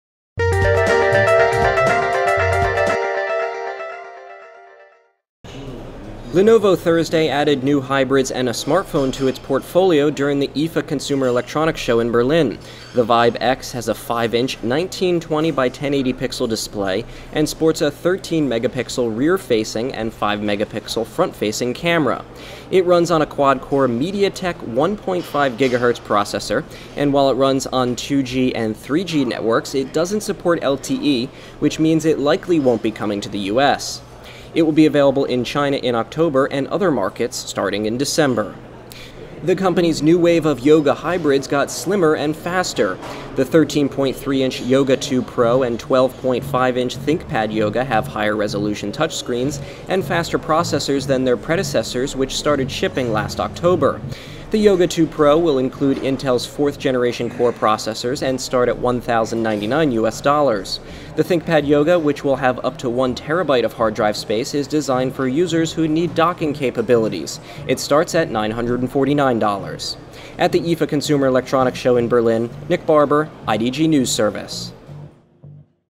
IFA 2013: Lenovo adds new hybrids, smartphone to portfolio
Lenovo added new hybrids and a smartphone, the Vibe X, to its portfolio during the IFA consumer electronics show in Berlin. Follow reporter Nick Barber on Twitter @nickjb